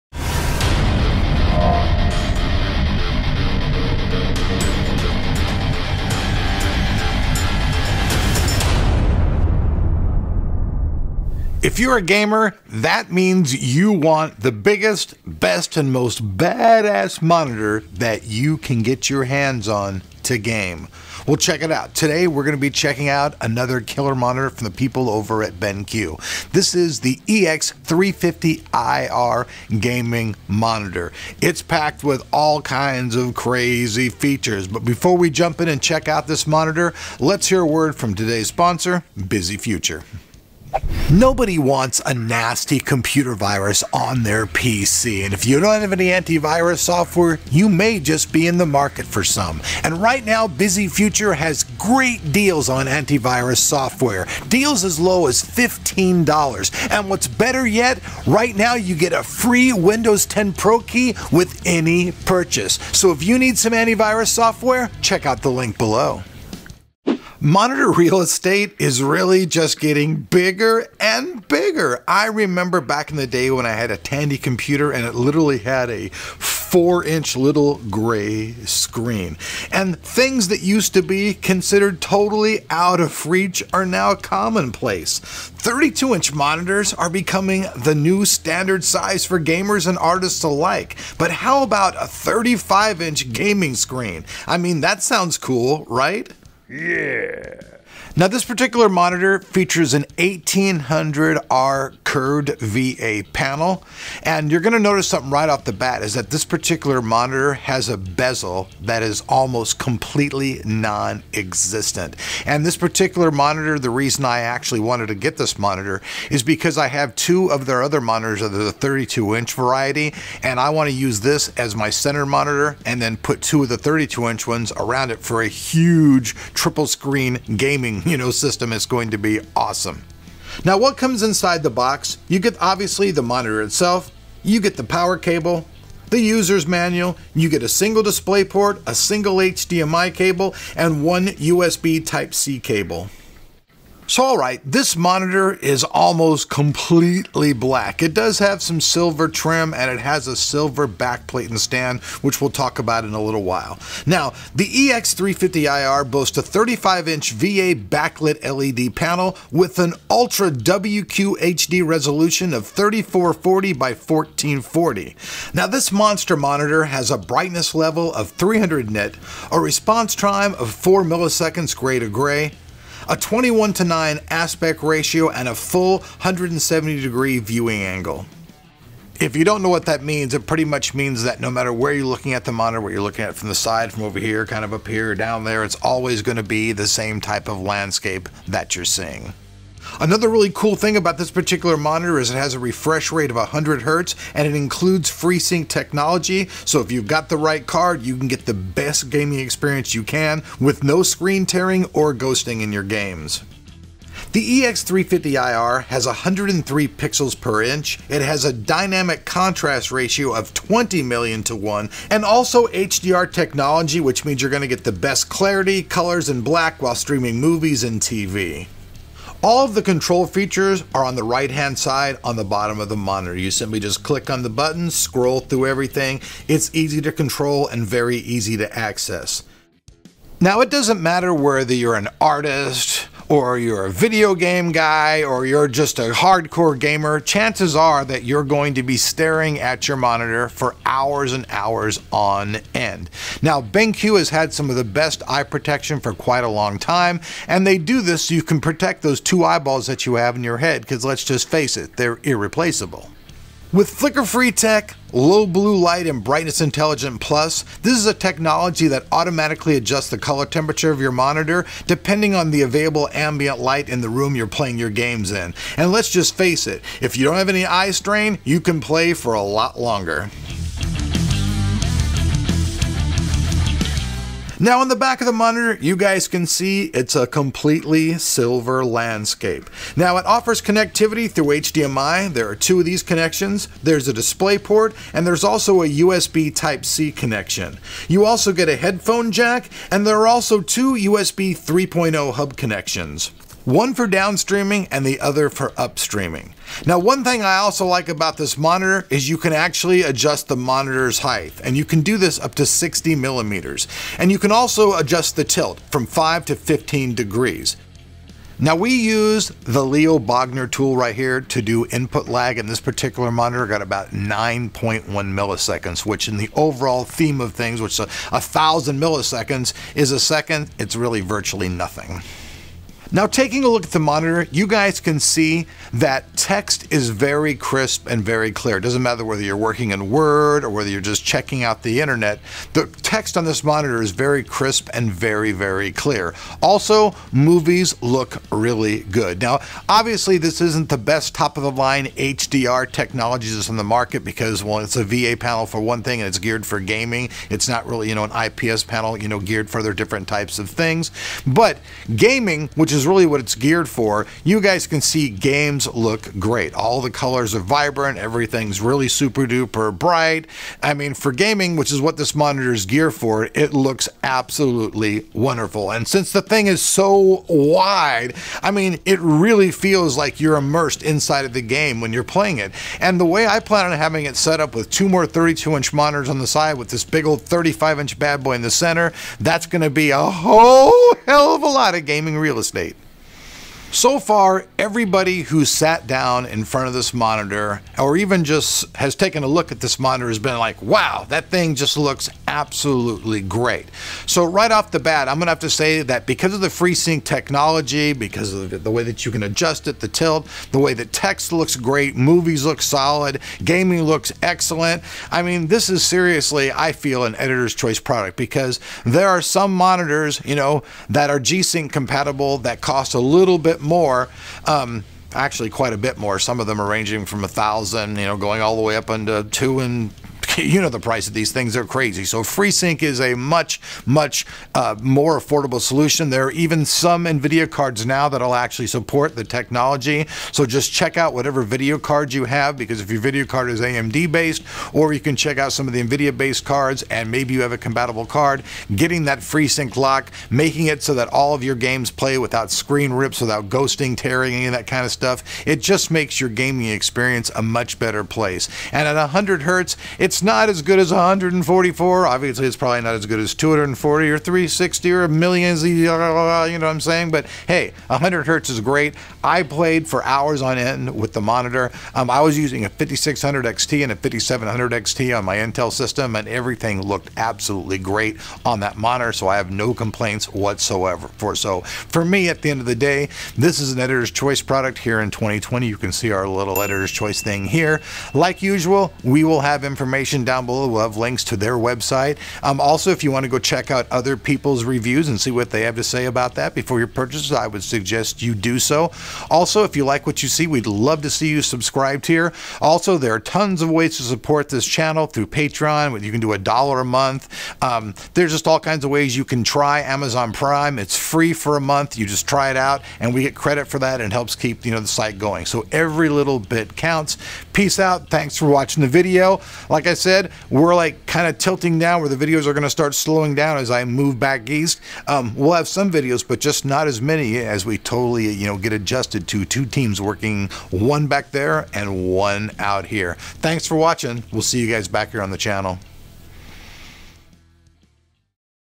The Best 35" Curved FreeSync Gaming Monitor?
The Best 35" Curved FreeSync Gaming Monitor? Looking for a ginormous gaming monitor for your gaming setup, check out this monster of a display. The BenQ EX3501R is 35 inch gaming monitor featuring Freesync, a 100Mhz Refresh rate, HDR and great eye care this is a serious gamers short list product. Watch the video and see just why!

BenQ EX3501R Ultrawide Curved QHD Monitor